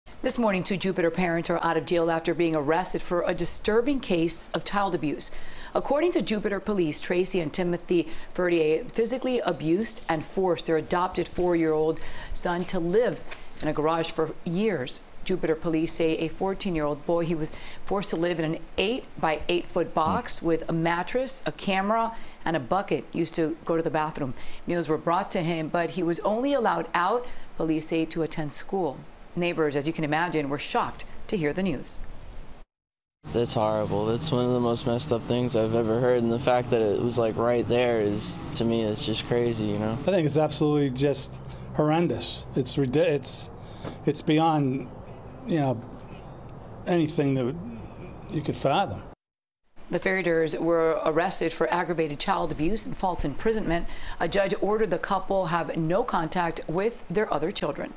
Florida Couple Accused Of Forcing Adopted Son To Live In Garage
Tracy and Timothy Ferriter reportedly abused the boy and forced him to live in a box in the garage. They have been charged with aggravated child abuse.